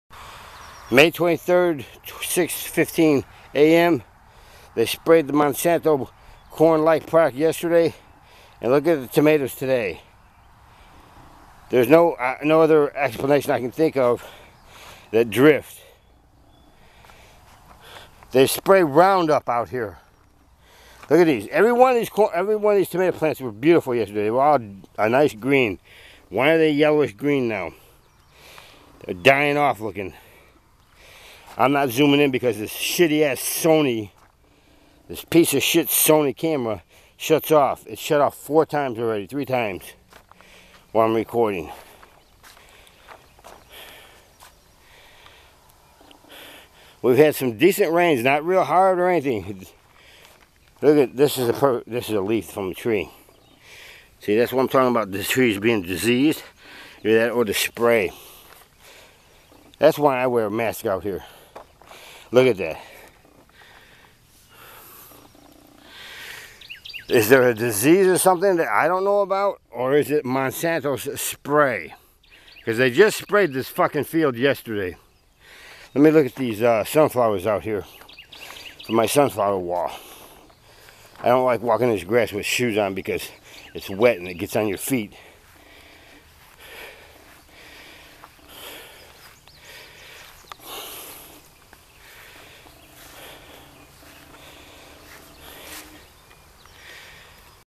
4 MONSANTO SPRAY
* MAY CONTAIN ADULT WORDS-THUS CENSORED BY YOU TUBE!
Wife woke me up in tears!  THEY  SPRAYED THEIR  MONSANTO FOOD LIKE  products YESTERDAY... and THE HEALTHY  HEIRLOOM VEGETABLES that WERE  out in garden ALL LOOK  as if they HAVE been SPRAYED! THEY  USE MONSANTO WHICH HAS DOWNPLAYED their  USE OF  roundup!  PROVEN TO CAUSE  CANCERS ! ! ! LOW VIEWS DUE TO THE YOUTUBE/MONSANTO  DEEP STATE, THESE TYPE OF VIDEOS ARE CONSTANTLY  SUPPRESSED !